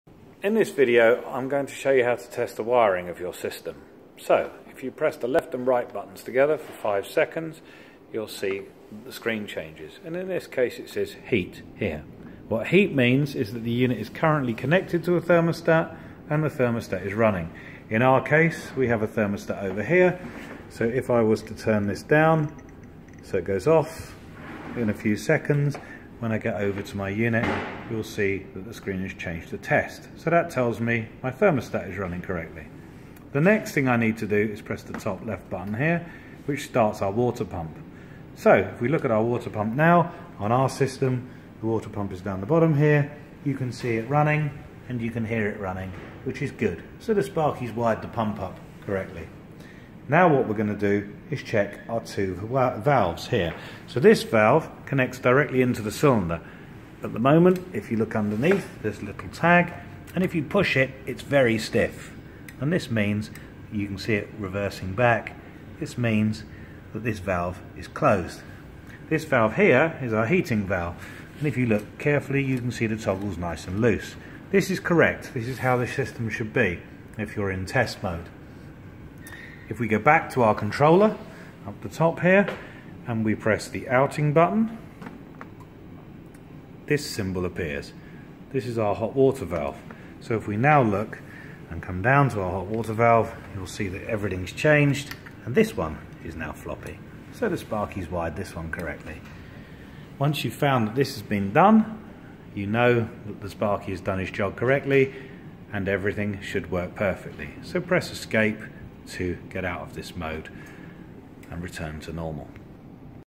testing the wiring on Samsung EHS heat pump
This video lets you check the wiring on your unit to see if its installed correctley